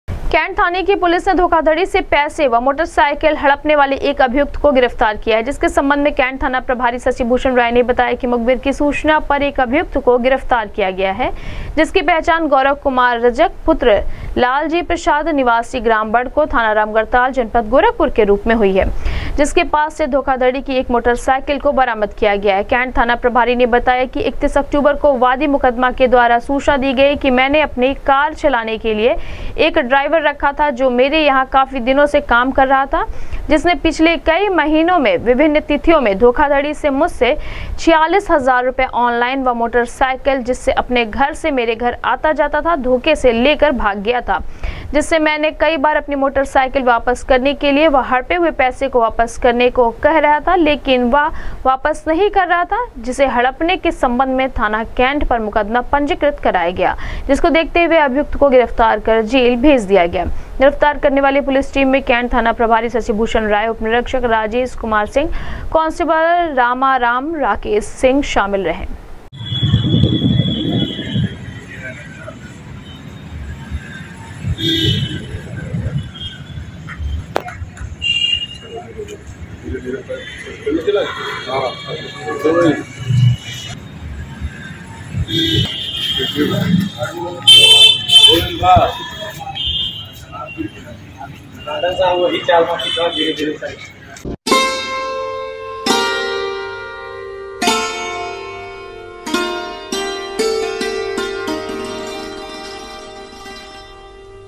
धोखाधड़ी से पैसे व मोटरसाइकिल हडपने वाले शातिर अभियुक्त गौरव कुमार रजक को कैंट थाने की पुलिस ने किया
धोखाधड़ी से पैसे व मोटरसाइकिल हडपने वाले शातिर अभियुक्त गौरव कुमार रजक को कैंट थाने की पुलिस ने किया गिरफ्तार, धोखाधड़ी से हड़पी हुई मोटरसाइकिल को किया बरामद,भेजा जेल